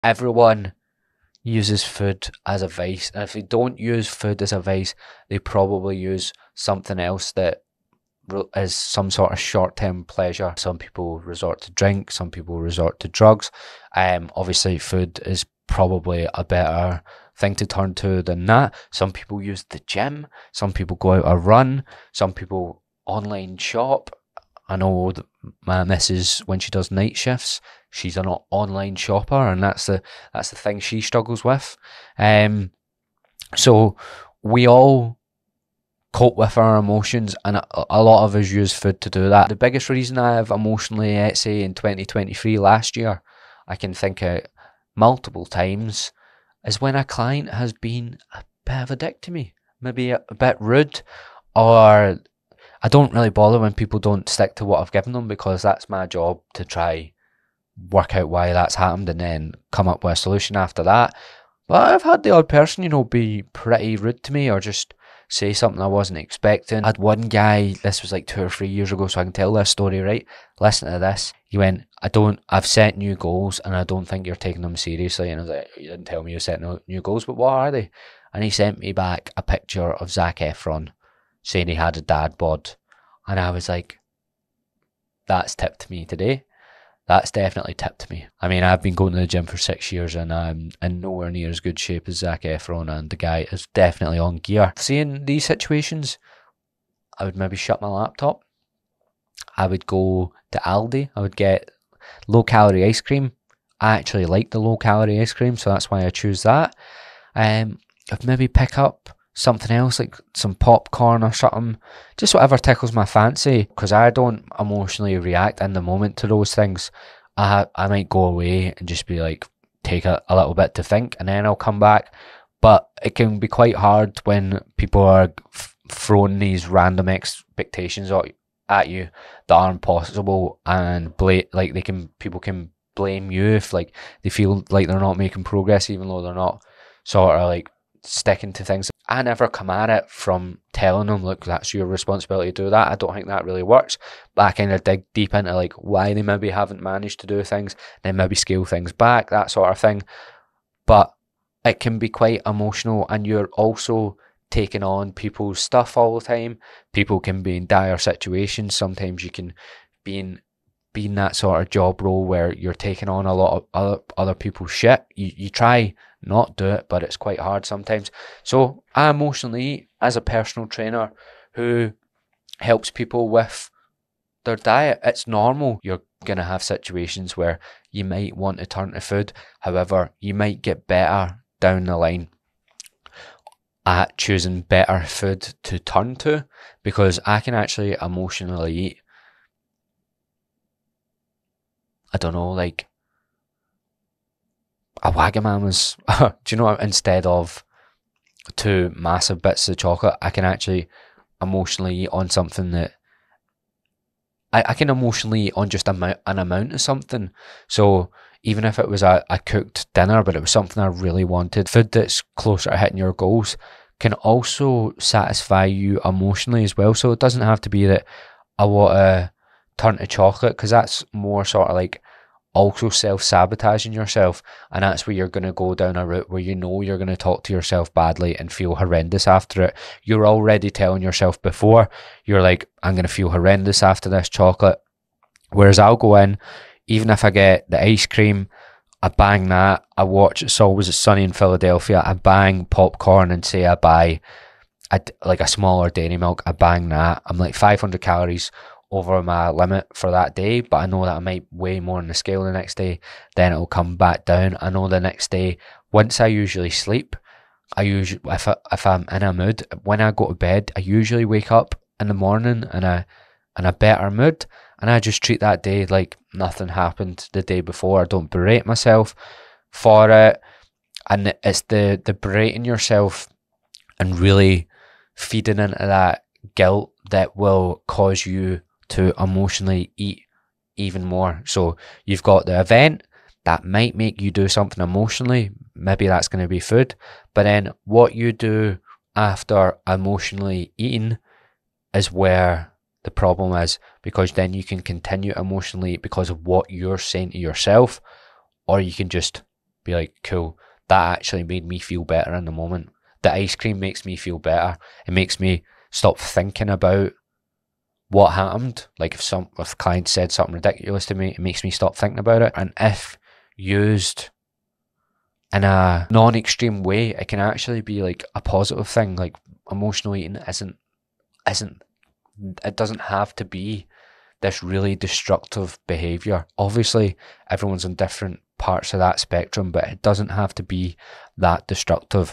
Personal Trainer Talks Openly About How He Emotionally Eats And How To Stop
Wether you have a weight problem or not or are trying to lose weight you will mostly likely having a coping mechanism that you might not be aware of. For a lot of us that coping mechanism when we feel stressed or emotional is often food and where a lot of 'binging' comes from.

I'm a personal trainer and I emotionally eat.. It is not something I try prevent it is something I try manage. Over eating is often a problem with thought patterns and how we speak to ourselves so how we frame 'binging' is extremely important. Since this is usually more emotional and it is impossible to never encounter anything in life that is hard to deal with you may always need coping mechanisms like food so in this podcast I try give you the tools to both stop over eating so much but also be okay with still using food to cope with your emotions because it's not as bad an idea as you think.